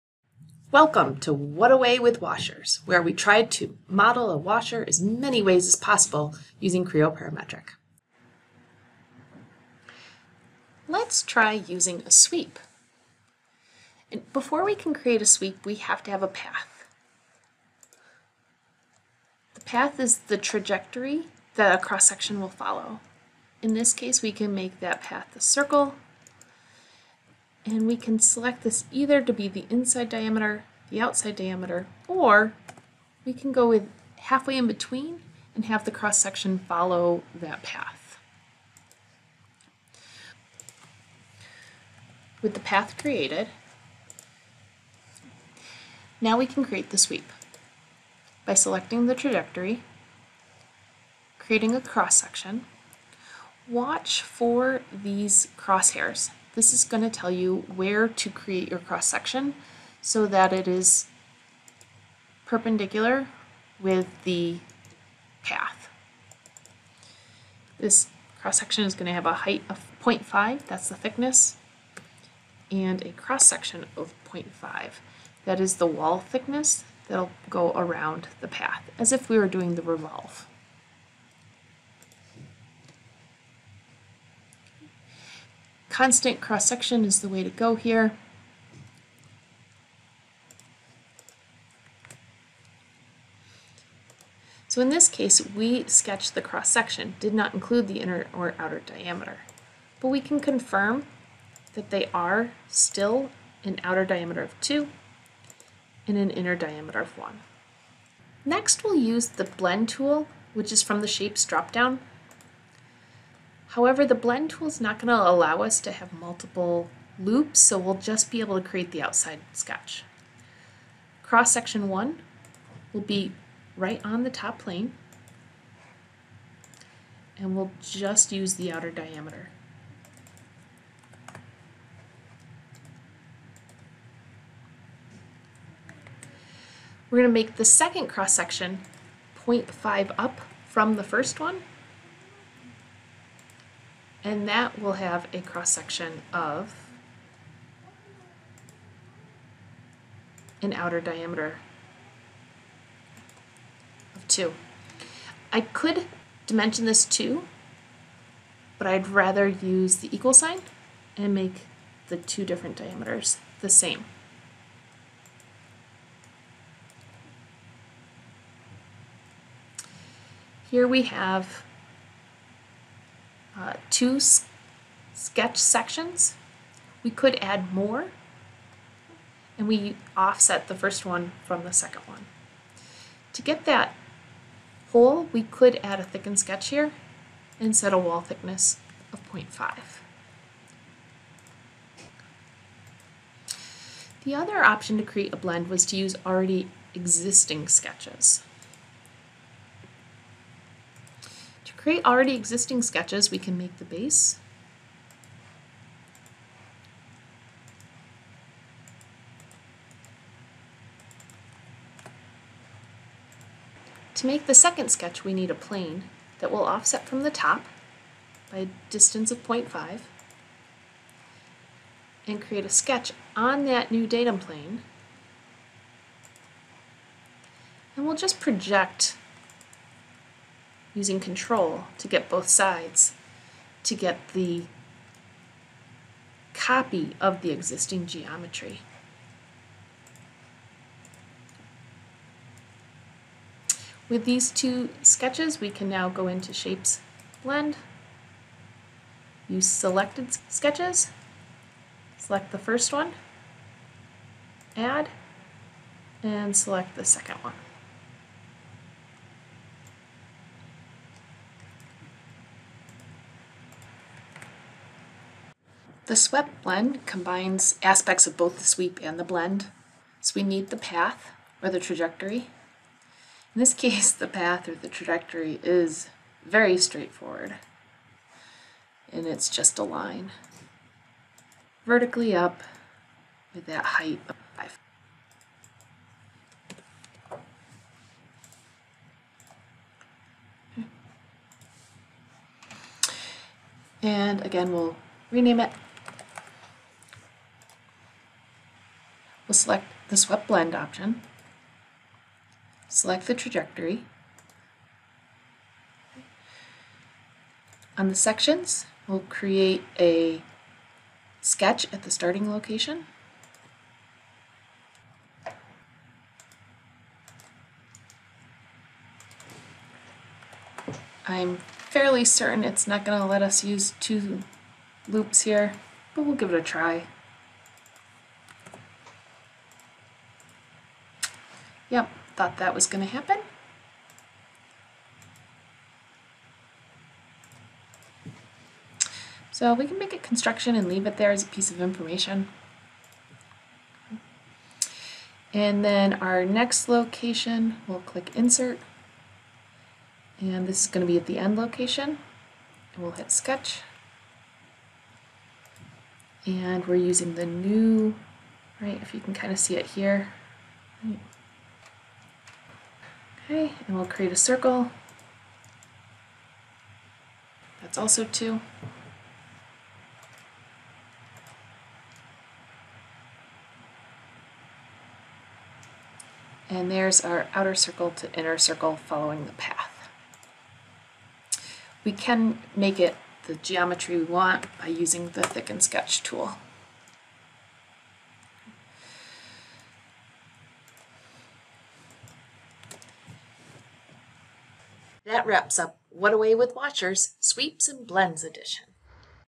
What a Way with Washers! Sweeps and Blends Edition
What a way with washers will investigate how many possible methods there are to model the simple shape of a washer using Creo Parametric.  This video series will demonstrate how to use many of Creo's feature tools applied to the simple washer shape.

0:09 - (15) - Using a sweep
1:48 - (16) - Creating a blend (loft) with sketched sections
3:20 - (17) - Creating a blend (loft) with selected sections
4:25 - (18) - Using a Swept Blend